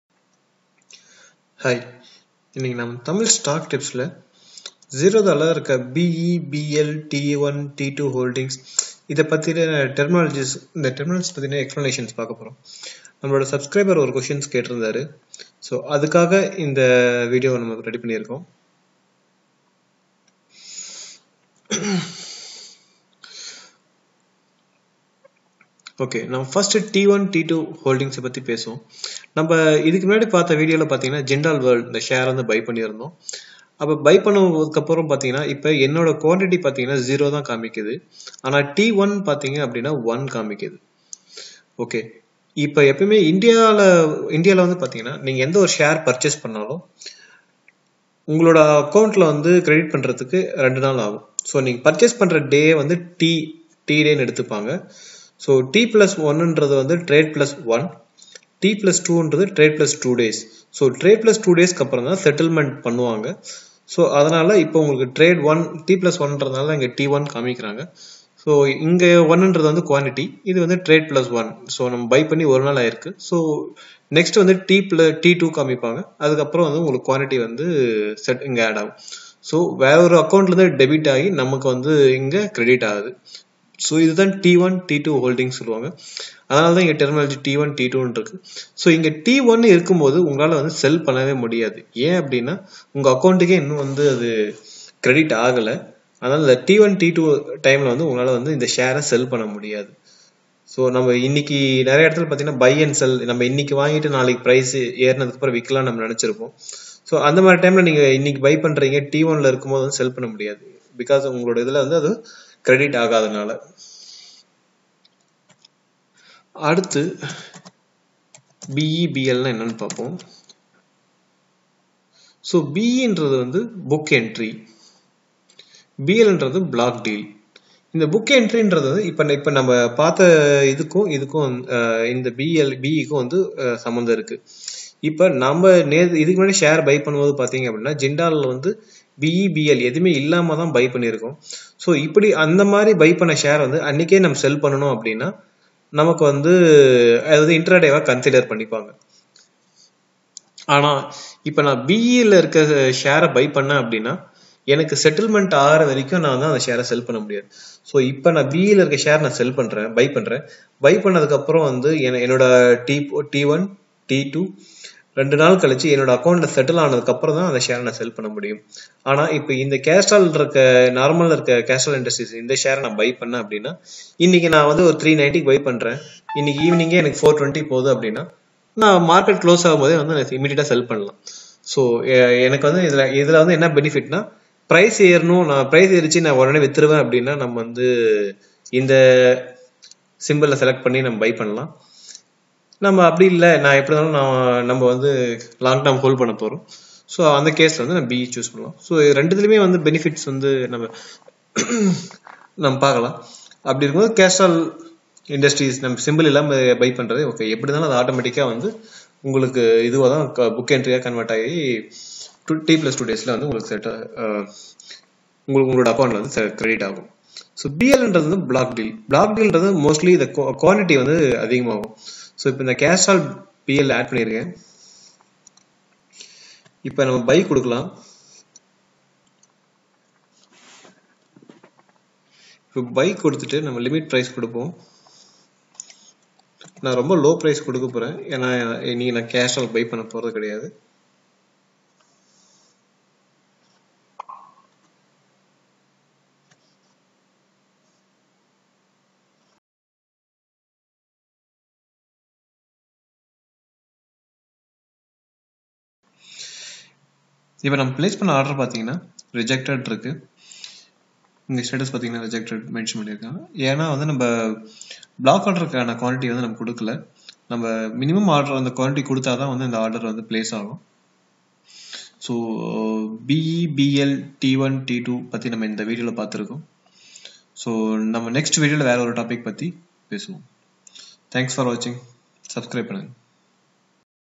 What is BE, BL, T1, T2 in Zerodha - Tamil
BE - Book Entry
BL - Block Deals
T1 & T2 - Holdings